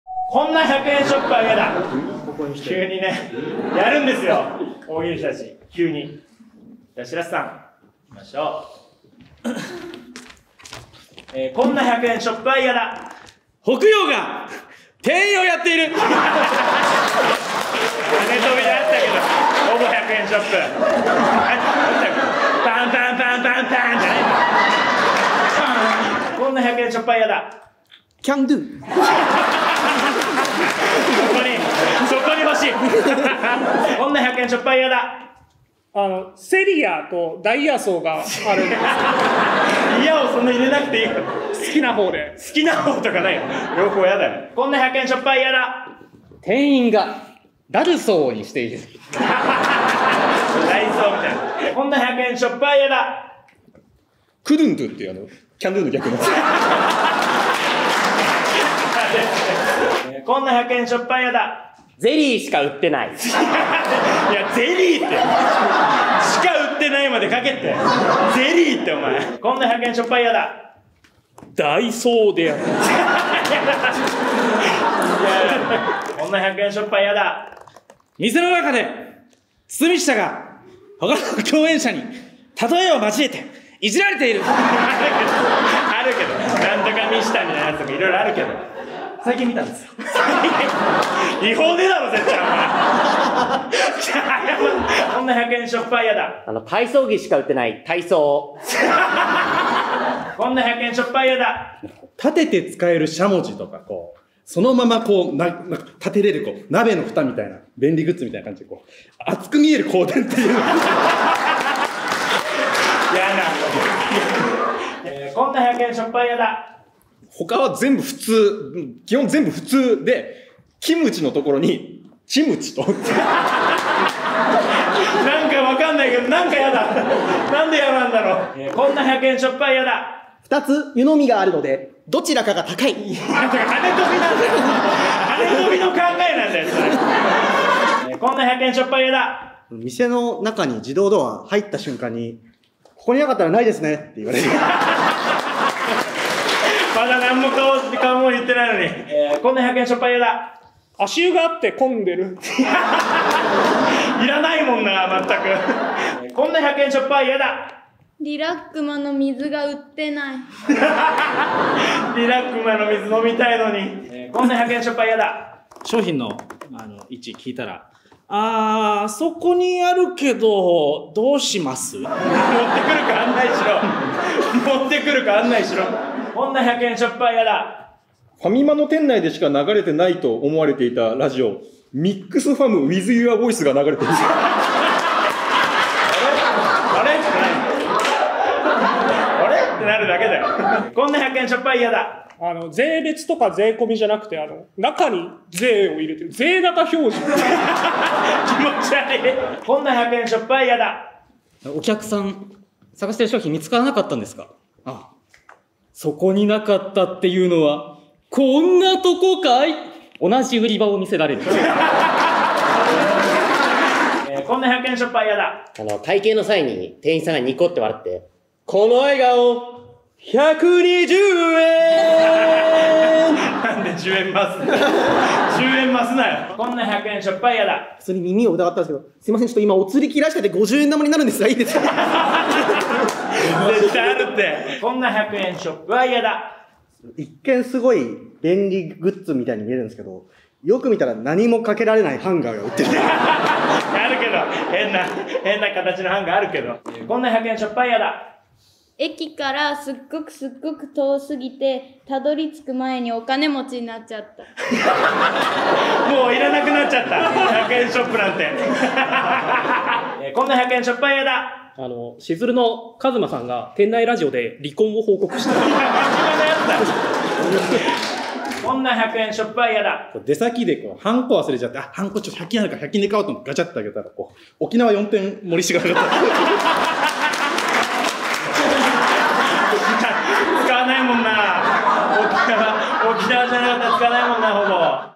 【大喜利】こんな100円ショップは嫌だ【大喜る人たち542問目】
2023/6/22に方南会館にて開催された「大喜る人たち オムレツ」より

大喜る人